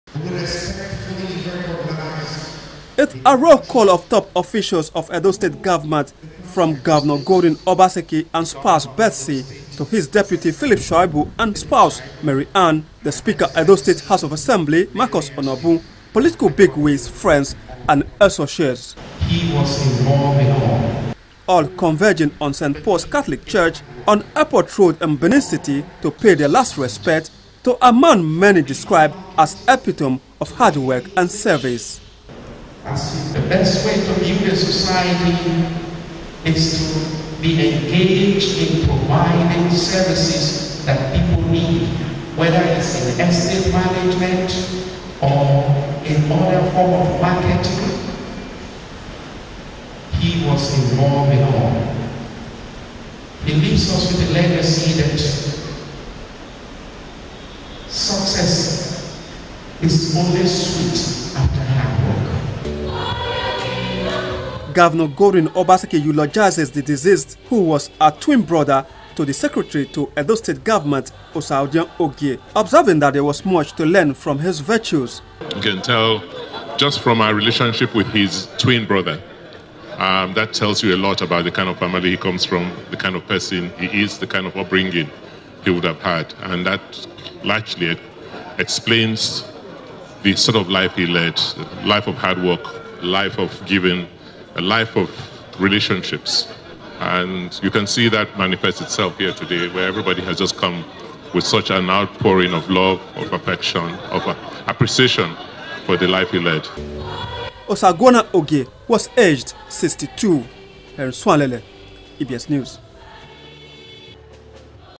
LATE OSAGUONA OGIE
Governor Obaseki Extols Virtues Of Deceased